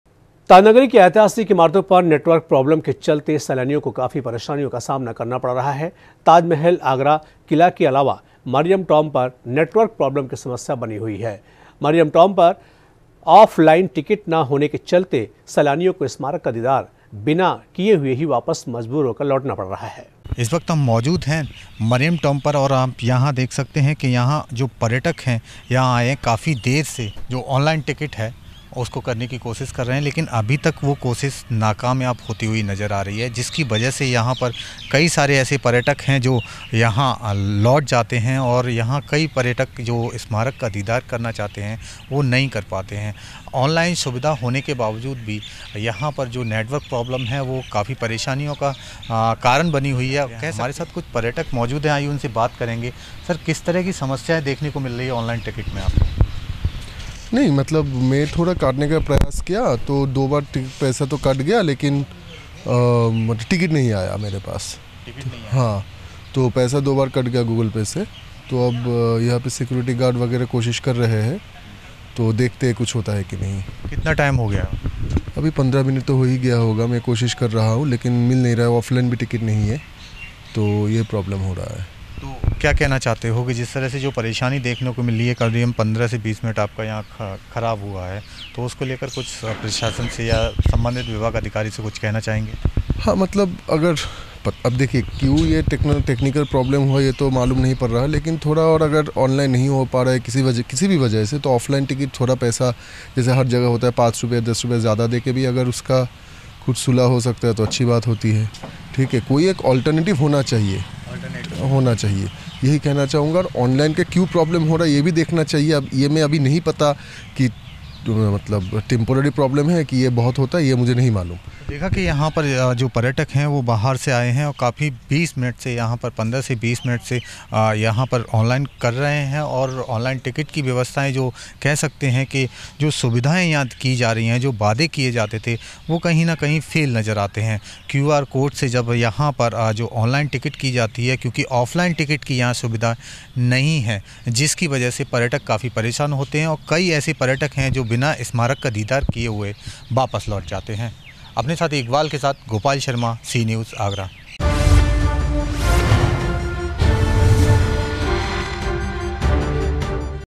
ऐतिहासिक इमारतों पर नेटवर्क प्रॉब्लम | Network problem on historic buildings
ताजनगरी की ऐतिहासिक इमारतों पर नेटवर्क प्रोब्लम के चलते सैलानियों को काफी परेशानी का सामना करना पड़ रहा है। ताजमहल,आगरा किला के अलावा मरियम टॉम पर नेटवर्क प्रोब्लम की समस्या बनी हुई है। मरियम टॉम पर आॅफ लाइन टिकट ना होने के चलते सैलानियों को स्मारक का दीदार किए बिना लौटने को मजबूर होना पड़ रहा है।